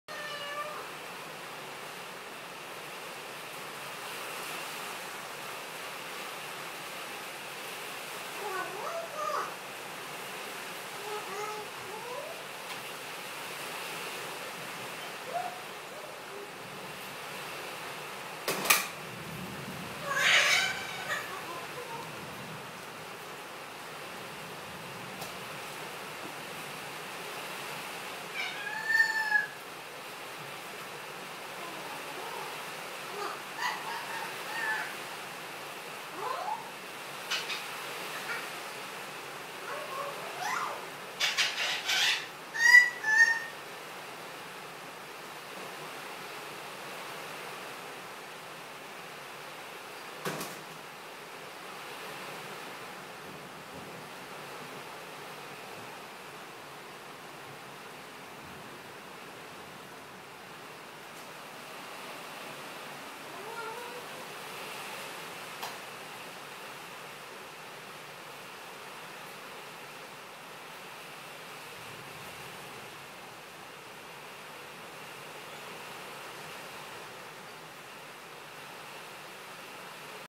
Ruby singing in the storm
Our 5 month old Eclectus parrot having a good chat in the storm. You can hear her meowing and laughing!